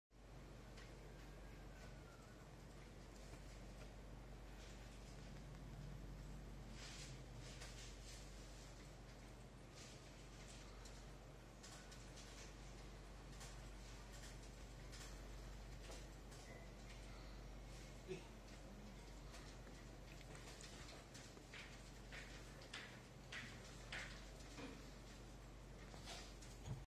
Edan, Perampok amatir ini bersenjata kunci Inggris tanpa penutup wajah
Perampokan yang dilakukan pria bersenjata kunci Inggris tanpa penutup wajah di Ketapang Kendal Kota Jawa Tengah, Kamis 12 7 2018 pagi